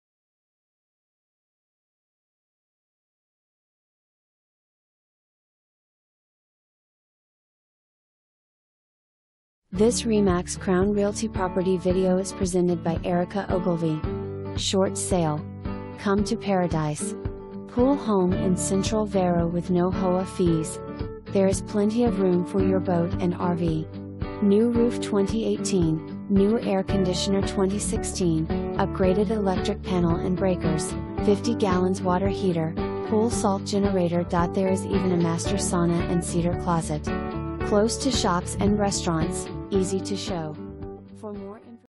RE/MAX Crown Realty - 1335 42nd Avenue
Please enjoy this property video created by RE/MAX Crown Realty

SHORT SALE. Come to paradise! Pool home in central Vero with NO HOA fees. There is plenty of room for your boat and RV. New roof 2018, new AC 2016, upgraded electric panel & breakers, 50 gal water heater, pool salt generator.There is even a master sauna and cedar closet. Close to shops and restaurants. easy to show!

2 full bath

Erica Ogilvie
RE/MAX Crown Realty